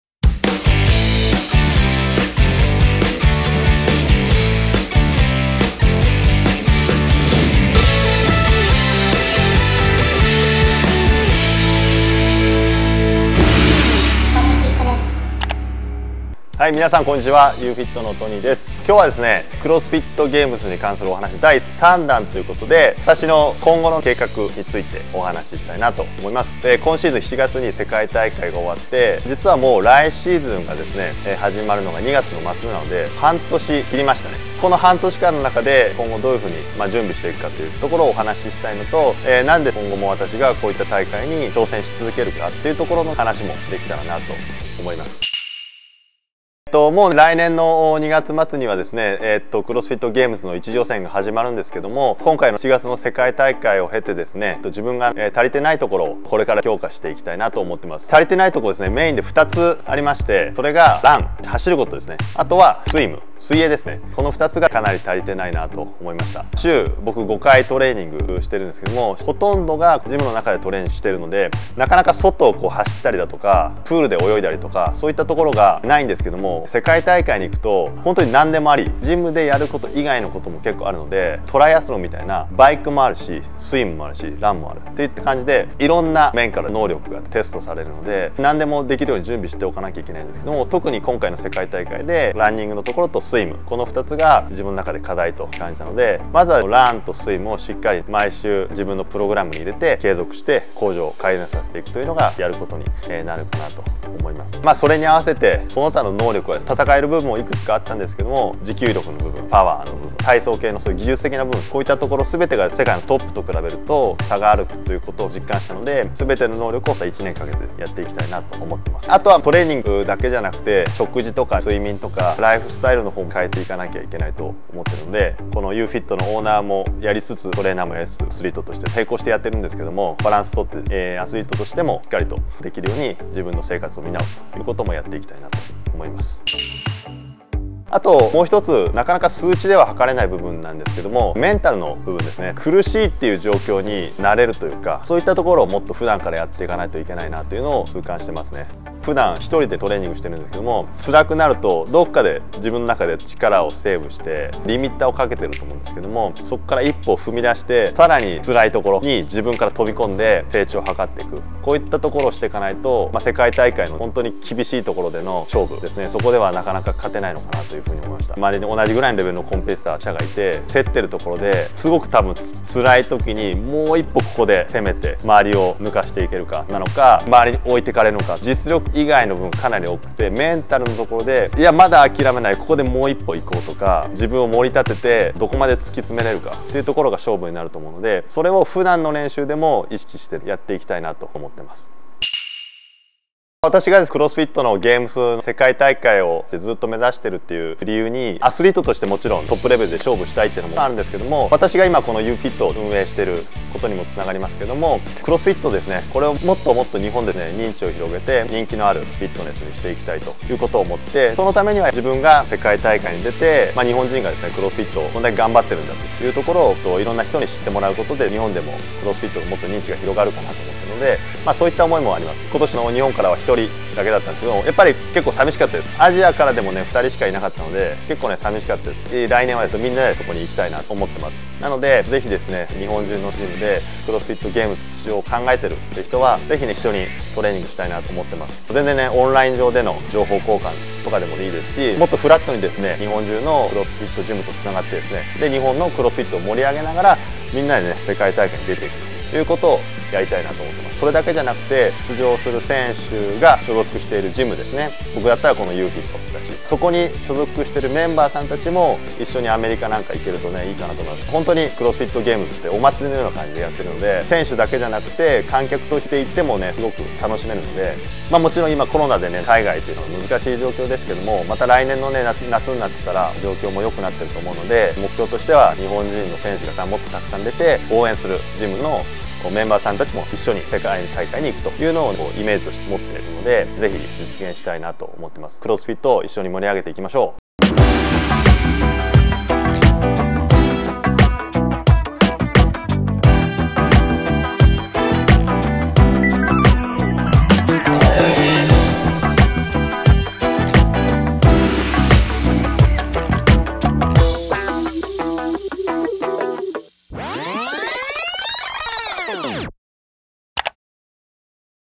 【CrossFit GAMES 2021 Talk vol.3】決意と使命・クロスフィットで日本を元気にしたい！｜横浜みなとみらい/CrossFit UNINTERRUPTED Fitness
0:00 オープニング
0:53 自分が足りていないところ
1:22 CrossFitには総合力が必要
2:00 食事、睡眠の改善
2:20 強いメンタル
3:11 CrossFitに対する想い
4:00 日本中のCrossFitを盛り上げたい
4:45 エンディング

無料体験レッスン、見学、ジムスペースレンタルのご相談